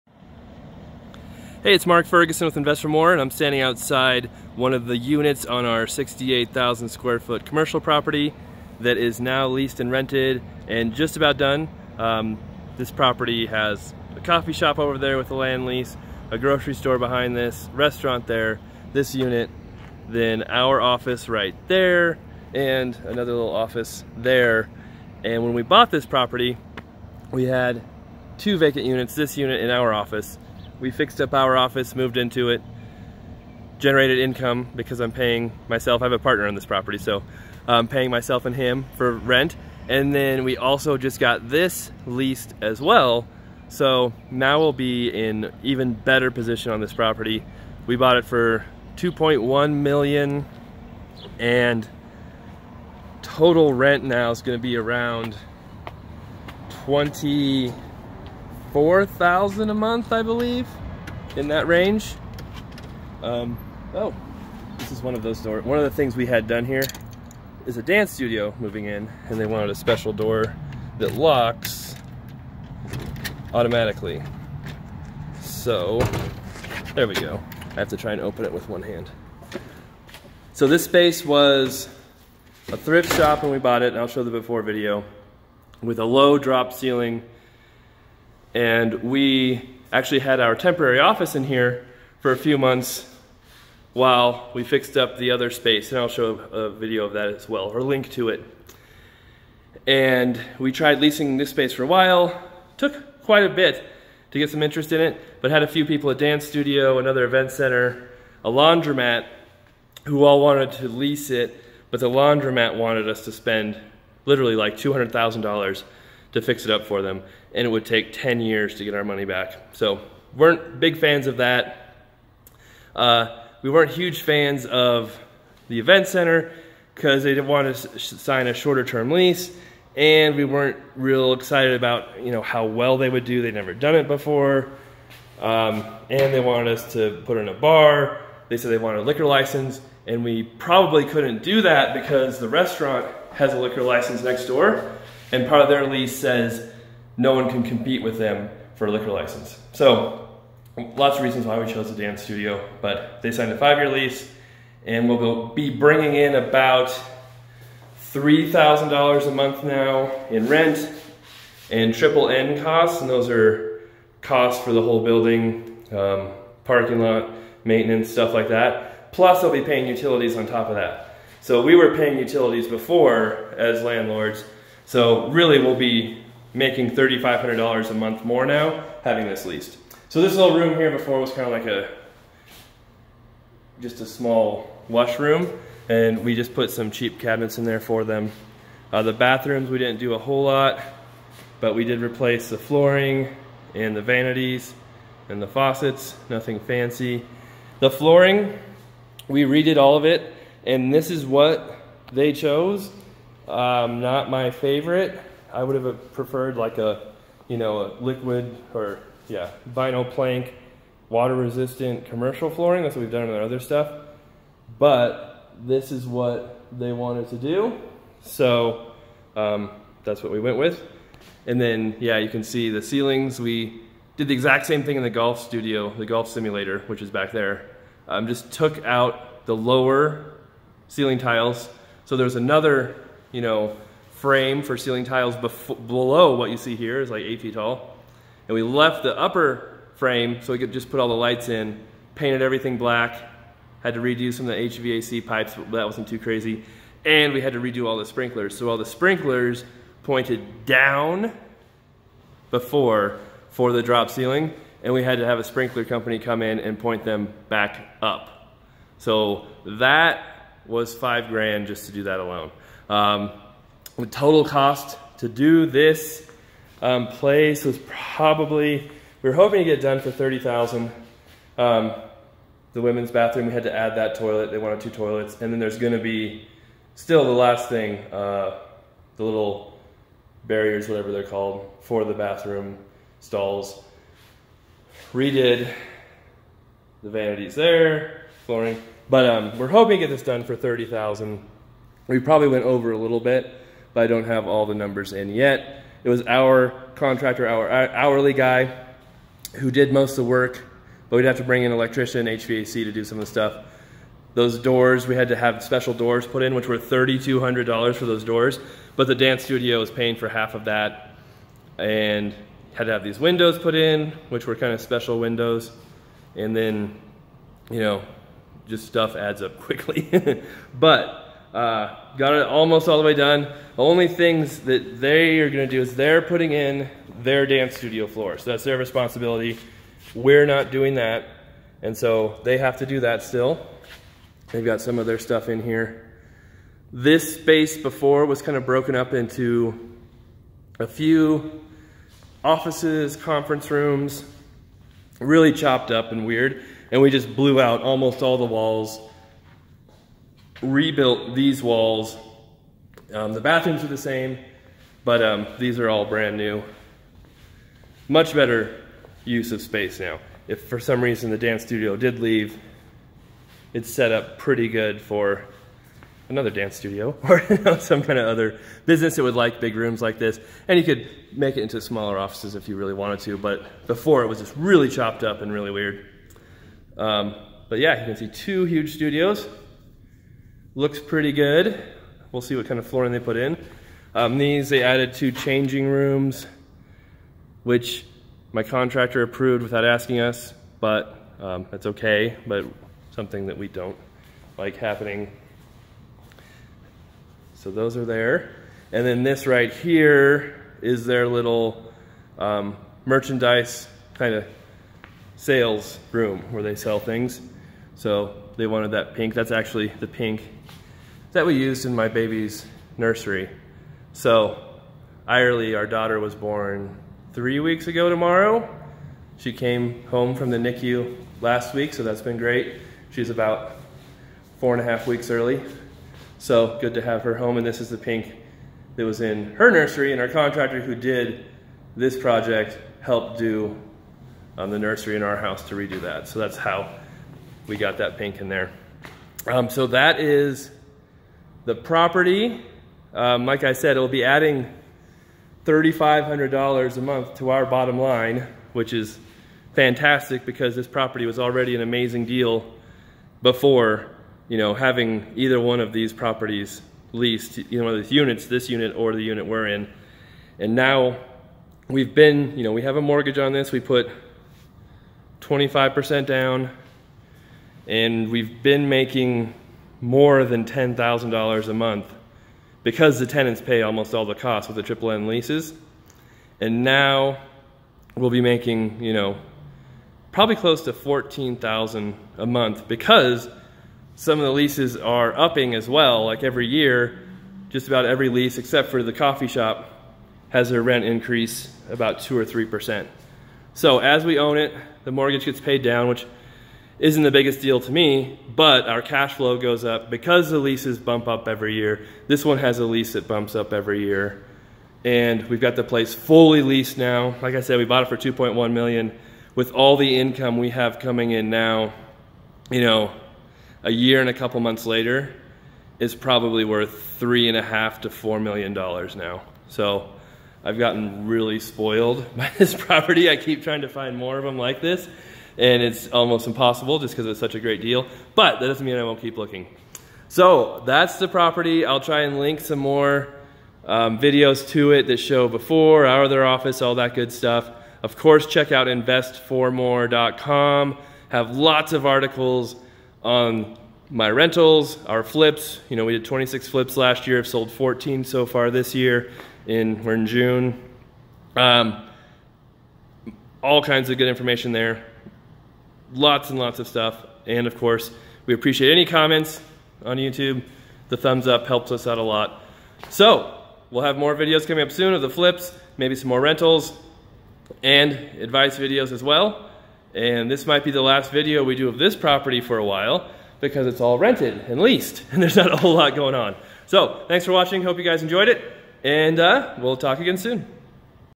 Last Unit Rented and Remodeled in My 68,000 SQFT Commercial Building
I bought this building in February of 2018 for $2.1 million. It consists of a grocery store, restaurant, coffee shop, and office space. It had two vacant units and you can see the building when I first bought it here:

I put my office in one of the units which you can see here:

This unit was our temporary office for a few months while we remodeled our office. It was a mess when we first got it and gutted most of the space. We added two large studio rooms, tore out the dropped ceiling, moved the sprinklers, put in windows, a new door and made some other minor repairs.

We should be bringing in $3,3000 to $3,500 a month more with the lease, NNN, and utilities they will now be paying.